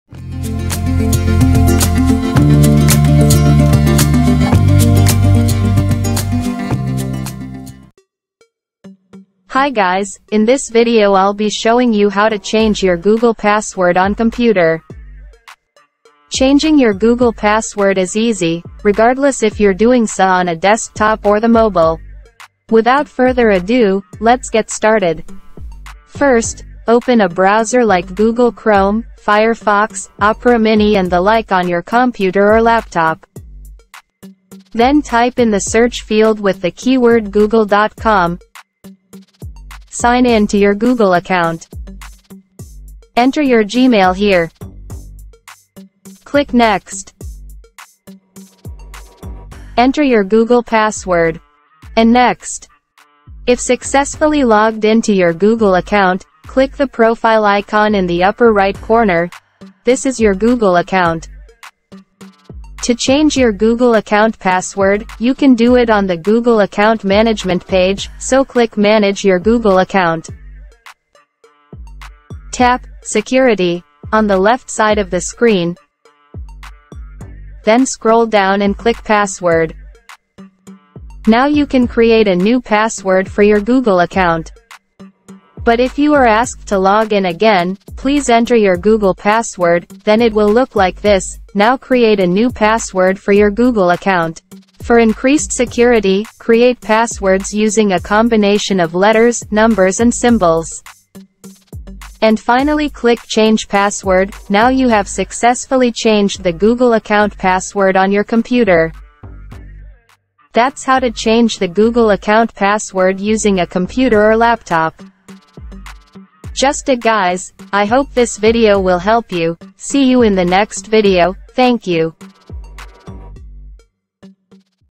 How to Change Google Password (on Computer or Laptop)
Hi guys, in this video i'll be showing you how to change google password  on computer or laptop. Changing your google password is easy, regardless if you’re doing so on a desktop or the mobile.

First, open a browser then type in the search field with the keyword google.com, sign in to your google account.

enter your gmail here. click next. enter your google password. and next. if successfully logged in to your google account, click the profile icon in the upper right corner, this is your google account.

to change your Google account password, you can do it on the Google account management page, so click Manage your Google account.

tap "security" on the left side of the screen, then scroll down and click password. now you can create a new password for your google account.

But if you are asked to log in again, please enter your google password, now create a new password for your Google account.

for increased security, create passwords using a combination of letters, numbers and symbols.

and finally click change password, now you have successfully changed the google account password on your computer.

That's how to change the Google account password using a computer or laptop.

google password.